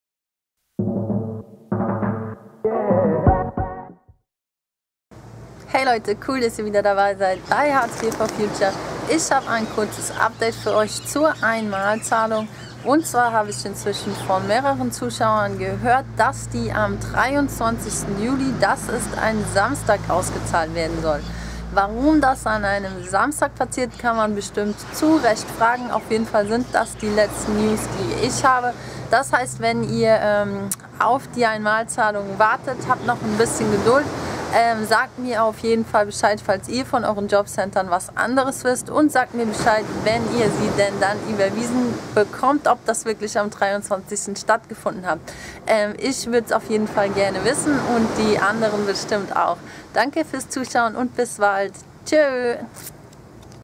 Wann kommt die Einmalzahlung??? --- UPDATE ---
Mehrere JC versprechen, dass die Einmalzahlungen am Sa. den 23.07. rausgehen. Hat jemand was anderes gehört? Und wann wird das Geld dann effektiv da sein?